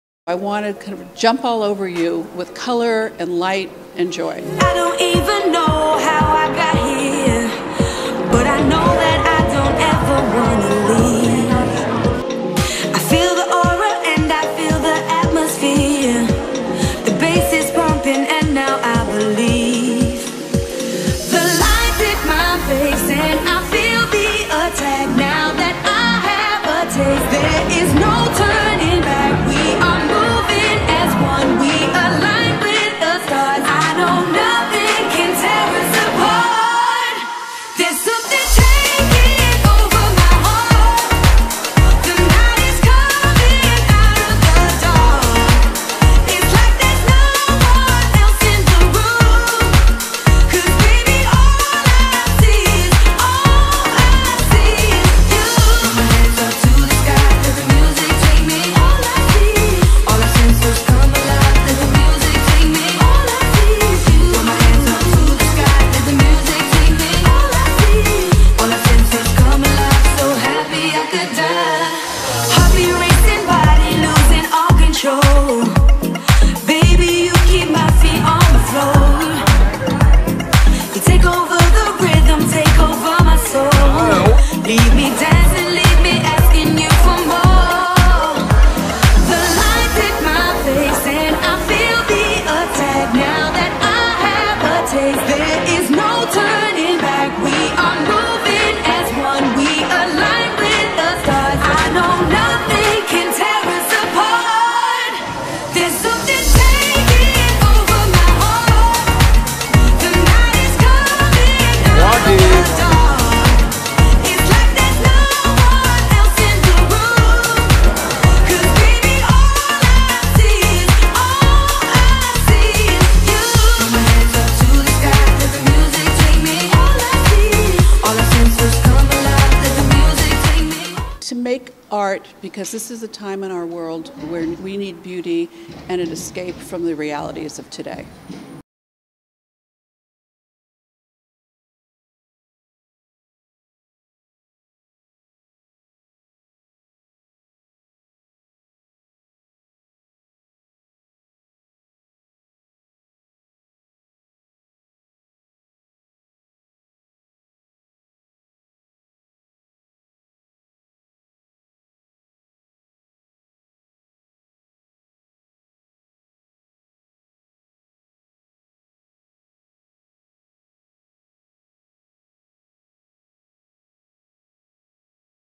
Allyson Taylor MRG Art Open w/ M. Goodman & T. Porter
MRG Fine Art Gallery Presents Textile Graffiti Paintings by Allyson Taylor MRG Fine Art Gallery Open w/ Michael Goodman & Tom Porter.Special thanks to Pasqual Bettio F.R.P.S. EYE on the ARTS!